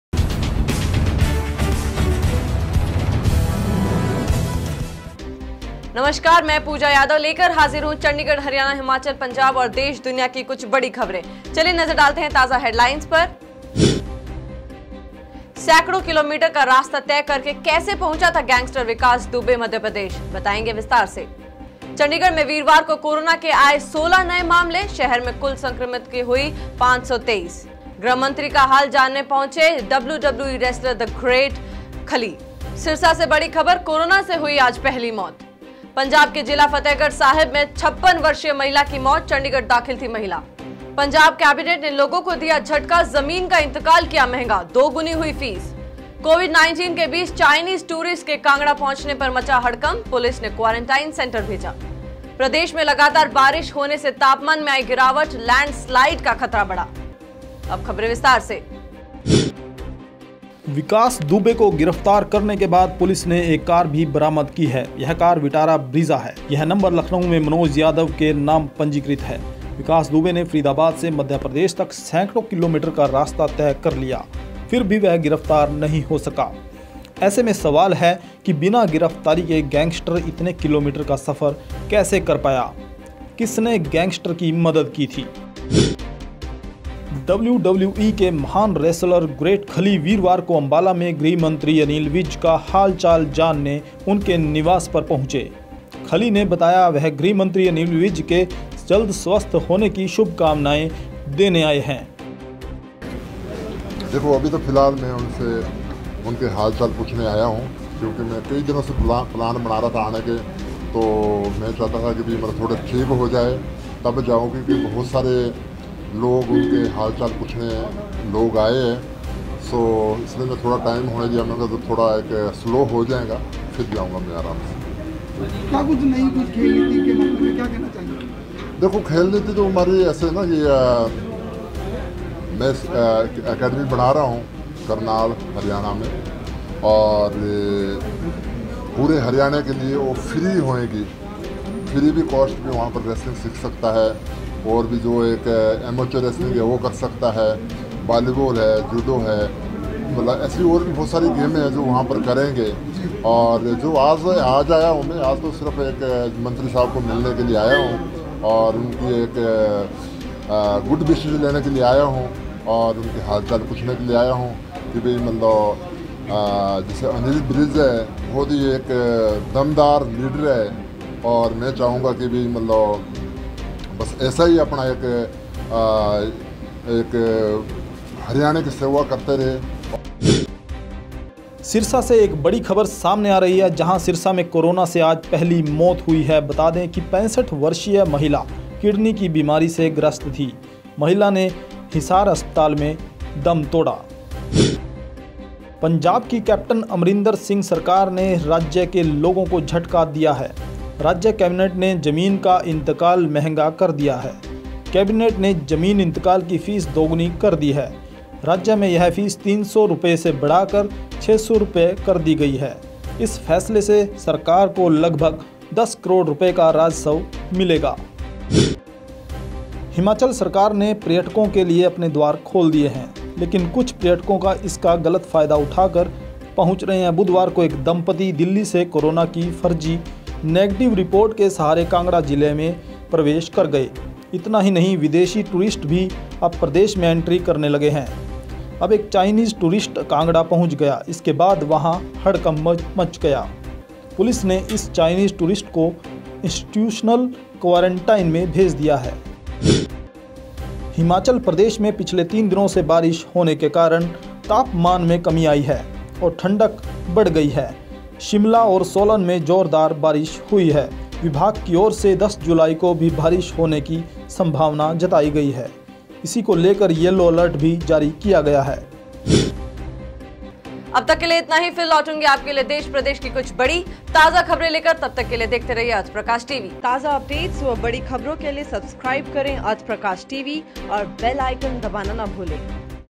ख़बरें Superfast - सैंकड़ों किमी का रास्ता तय करके कैसे पहुंचा था गैंगस्टर विकास दुबे MP, देखें
विकास दुबे को गिरफ्तार करने के बाद पुलिस ने एक कार भी बरामद की है।  यह कार विटारा ब्रीजा है। यह नंबर लखनऊ में मनोज यादव के नाम पर पंजीकृत है। विकास दुबे ने फरीदाबाद से मध्य प्रदेश तक सैकड़ों किमी का रास्ता तय कर लिया फिर भी वह गिरफ्तार नहीं हो सका। ऐसे में सवाल है कि बिना गिरफ्तारी के गैंगस्टर इतने किमी का सफर कैसे कर पाया? किसने गैंगस्टर की मदद की थी?

डब्ल्यूडब्ल्यूई के महान रेसलर ग्रेट खली वीरवार को अंबाला में गृह मंत्री अनिल विज का हाल-चाल जानने उनके निवास पर पहुंचे, खली ने बताया वह गृह मंत्री अनिल विज के जल्द स्वस्थ होने की शुभकामनाएं देने आए हैं

सिरसा से एक बड़ी खबर सामने आ रही है जहां सिरसा में कोरोना से आज पहली मौत हुई है। बता दें कि 65 वर्षीय महिला किडनी की बीमारी से ग्रस्त थी । महिला ने हिसार अस्पताल में तोड़ा दम।

पंजाब की कैप्‍टन अमरिंदर सिंह सरकार ने राज्‍य के लोगों काे झटका दिया है। राज्‍य कैबिनेट ने जमीन का इंतकाल महंगा कर दिया है। कैबिनेट ने जमीन इंतकाल की फीस दोगुनी कर दी है। राज्य में यह फीस 300 रुपये से बढ़ाकर 600 रुपये कर दी गई है। इस फैसले से सरकार को लगभग 10 करोड़ रुपये का राजस्व मिलेगा।

हिमाचल सरकार ने पर्यटकों के लिए अपने द्वार खोल दिए हैं, लेकिन कुछ पर्यटक इसका गलत फायदा उठाकर पहुंच रहे हैं। बुधवार को एक दंपती दिल्ली से कोरोना की फर्जी निगेटिव रिपोर्ट के सहारे कांगड़ा जिले में प्रवेश कर गए। इतना ही नहीं विदेशी टूरिस्ट भी अब प्रदेश में एंट्री करने लगे हैं। अब एक चाइनीज टूरिस्ट कांगड़ा पहुंच गया। इसके बाद वहां हड़कंप मच गया। पुलिस ने इस चाइनीज टूरिस्ट को इंस्टीट्यूशनल क्वारैंटाइन में भेज दिया है।

प्रदेश में पिछले तीन दिनों से बारिश होने के कारण तापमान में कमी आई है और ठंडक बढ़ गई है। शिमला और सोलन में जोरदार बारिश हुई है। विभाग की ओर से 10 जुलाई को भी भारी बारिश होने की संभावना जताई गई है। इसी को लेकर येलो अलर्ट भी जारी किया गया है।